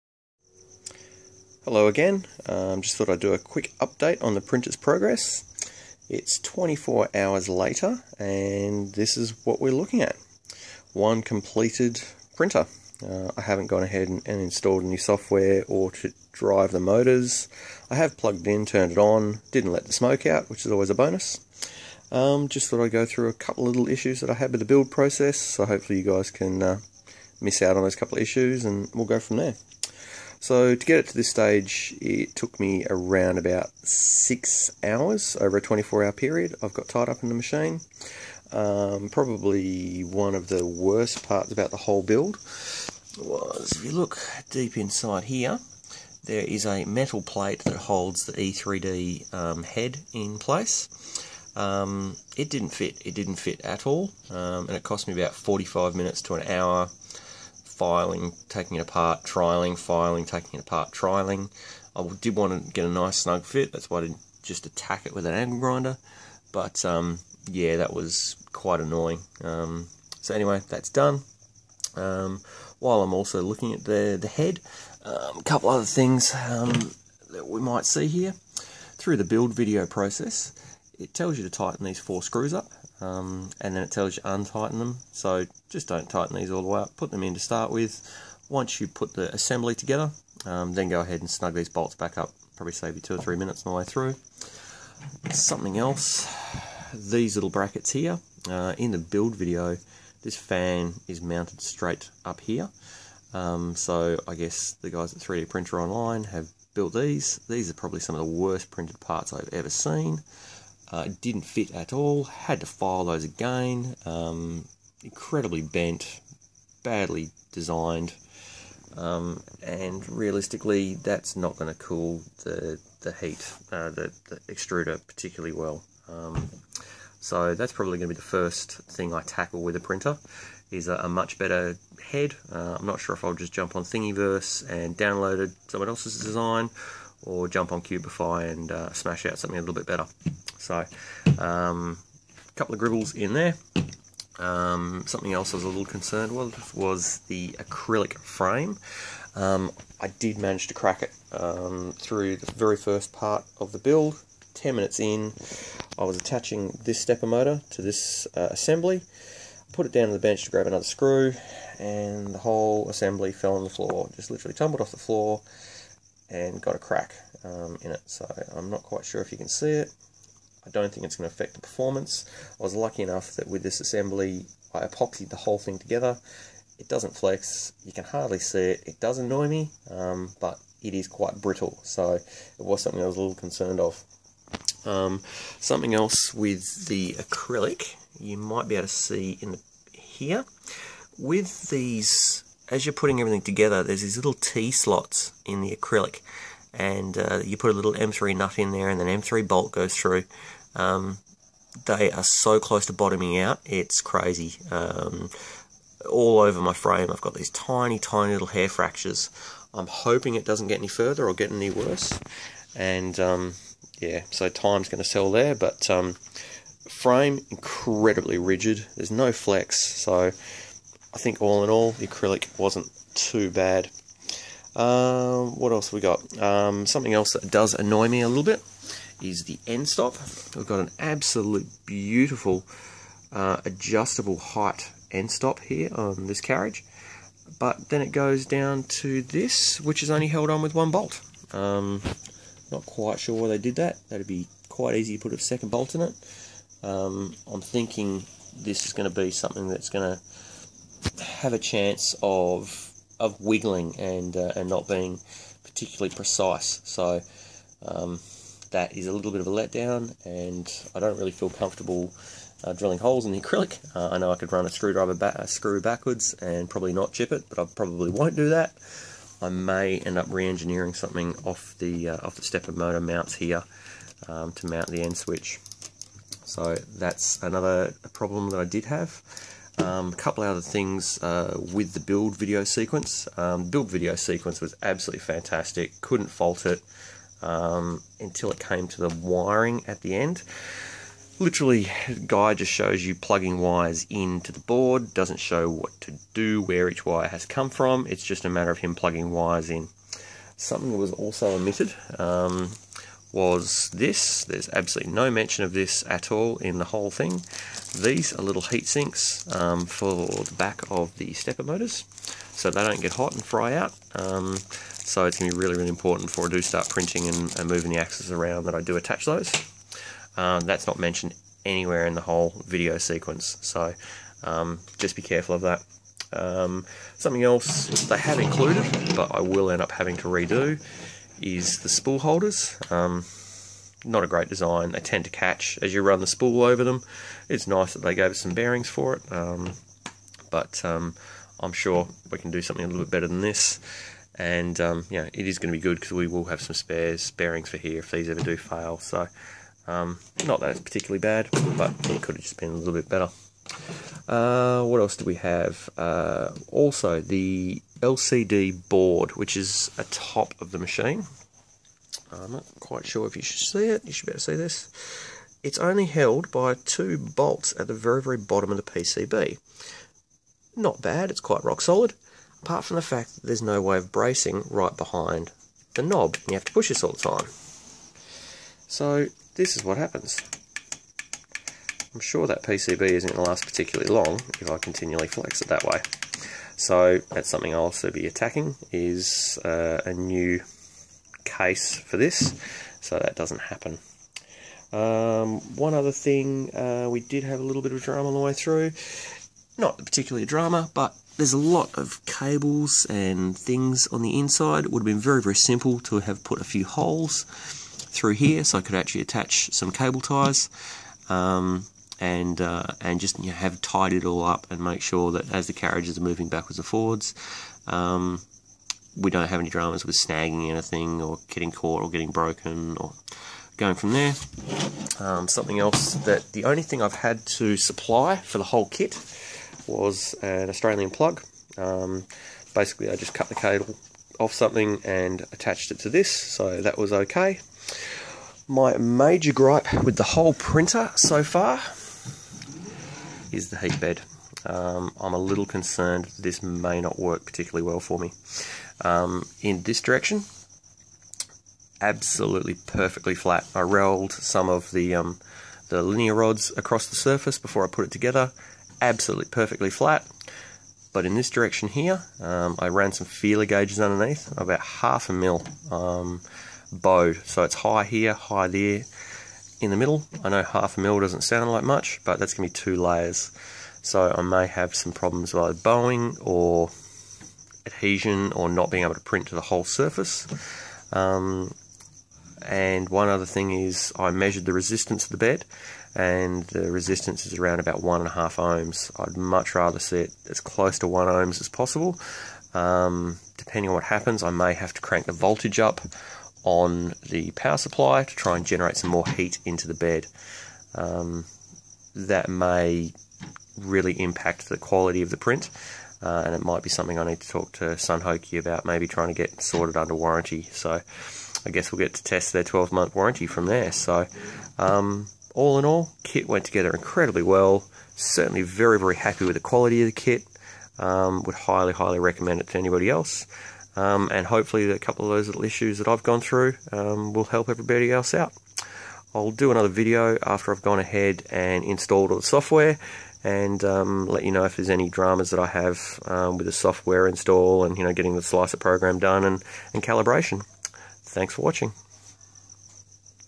Sunhokey prusa i3 3d printer rep rap kit build update 1
so the printer is together,
just wanted to talk about a few problems with the printer and build so far
so far so good very happy with purchase
hope the bed works well!